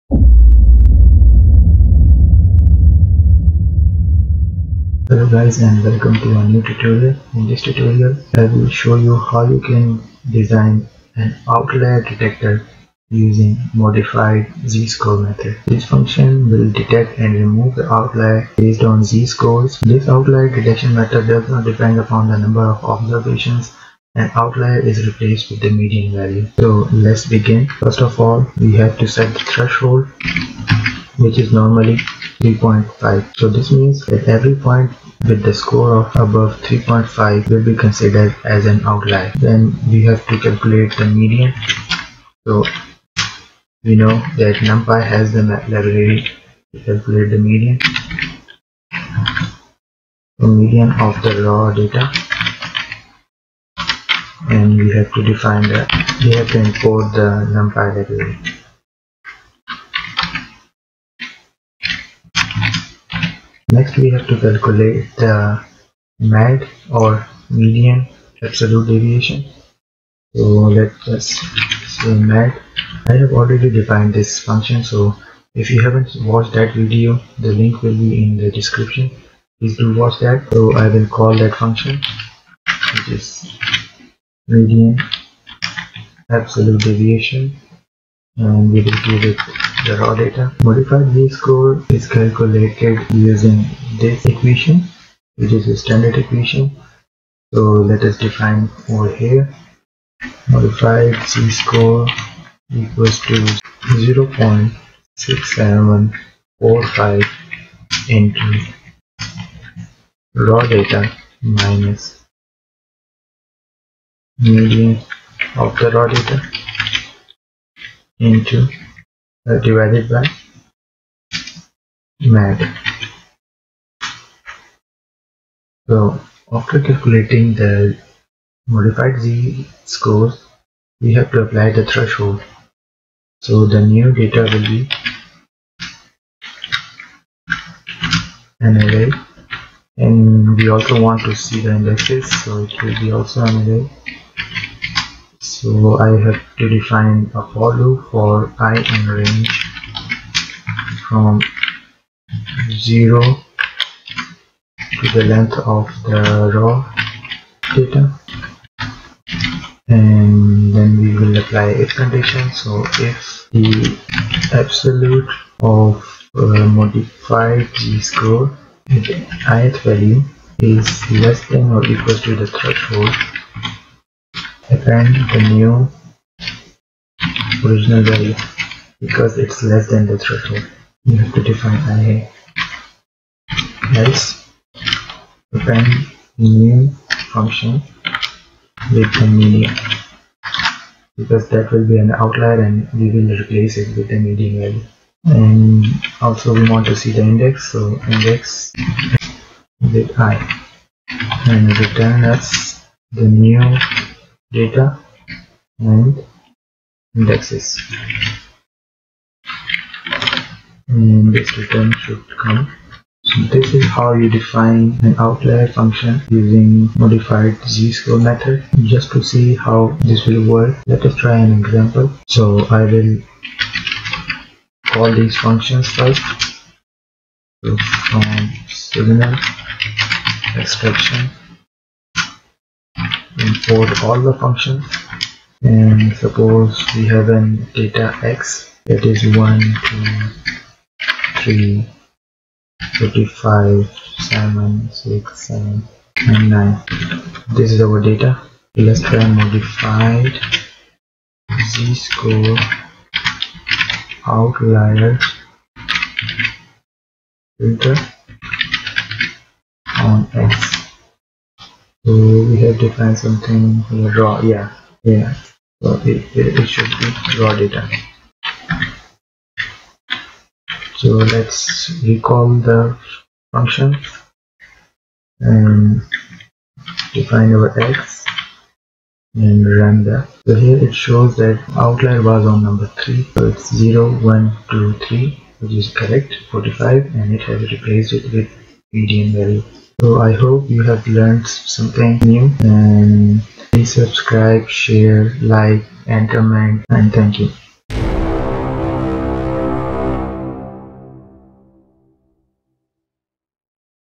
Modified Z-score Method for outlier detection and removal in Python
Note: Please Mute (Sound Off) the background Music.

This function detects and removes outliers based on Iglewicz and Hoaglin outlier tests. An outlier detection method that does not depend on the number of observations and outliers is replaced with median value.  We can calculate the Modified Z-score like: (0.6745*(raw_data - median(raw_data)))/MAD.  0.6745 is the 0.75th quartile of the standard normal distribution, to which the MAD converges to.  The threshold is generally taken at 3.5. This means that every point with a score above 3.5 will be considered an outlier.
MAD (median absolute deviation) function: (at 2:50)

This Channel helps in teaching how to do different projects in Proteus, Matlab, Python, and Shows different simulations of technical Projects.